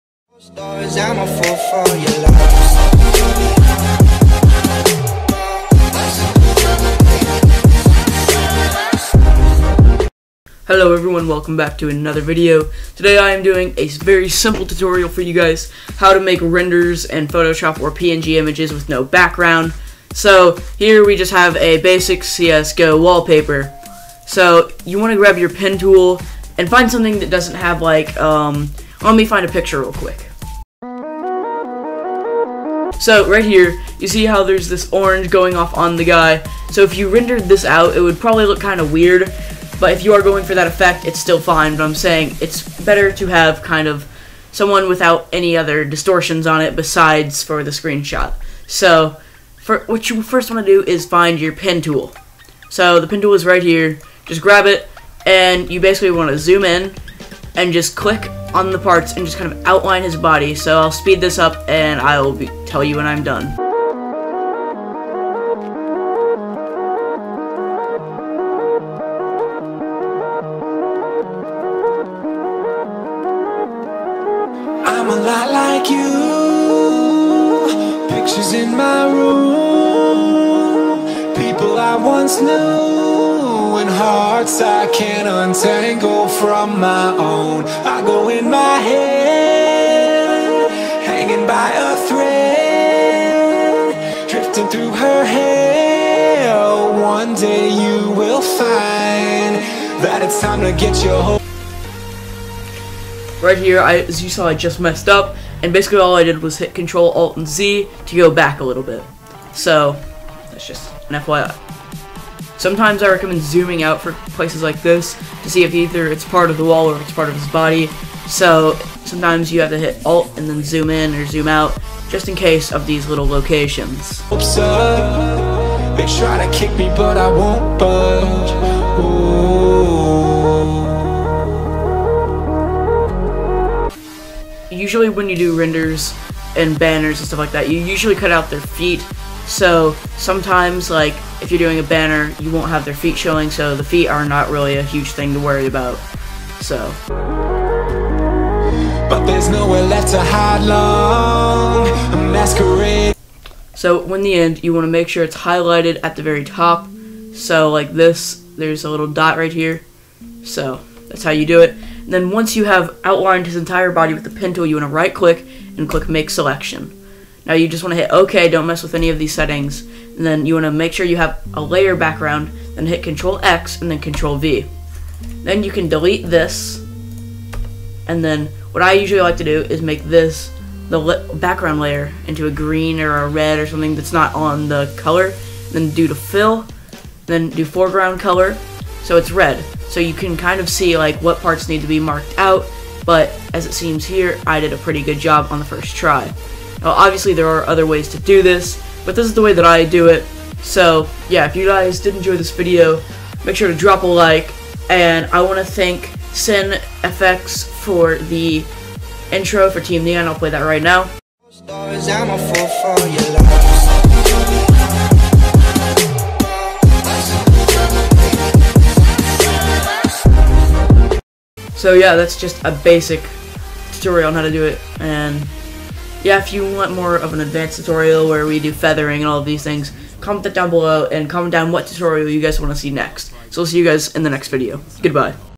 How To Render In Photoshop CC 2016 [TUTORIAL]
REMEMBER TO SAVE IT AS A PNG IMAGE!
How to make an image with a transparent background otherwise known as a render or a png

►STEAM TRADE URL

►Partner With Beast!